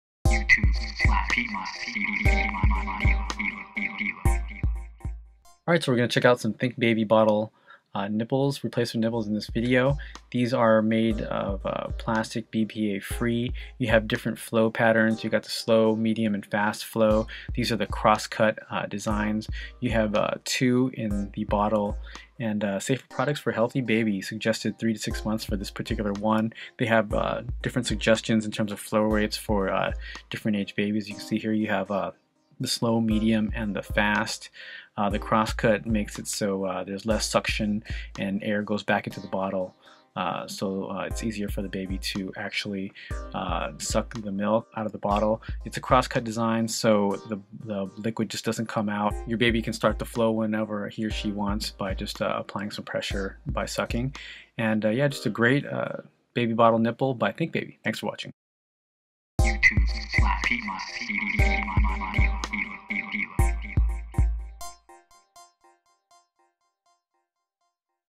ThinkBaby Bottle Nipples For Trainer Feeding Set Like Sippy Cups in Cross Cut Slow Med & Fast Flow
ThinkBaby bottle nipples for bottle feeding come in different flow rates for the various developmental stages / ages of your child which will help with feeding time.  The company is known for their baby products with innovative designs like the Think Baby Sippy Cup product line and feeding sets.  The cross cut feature as well as small, medium and fast flow rates make this one of the best baby bottle nipples for sale.  There are 2 to the set and used as replacements and additions to their bpa free baby bottles for feeding milk / formula and over liquids to babies.  If you're looking for something different from products by companies like Medela, earth Mama, Event, Nuk or Gerber, check out ThinkBaby bpa free baby bottles and baby bottle nipples in the various flow rates in the cross cut design and see if you're child prefers this design for bottle feeding with milk/formula.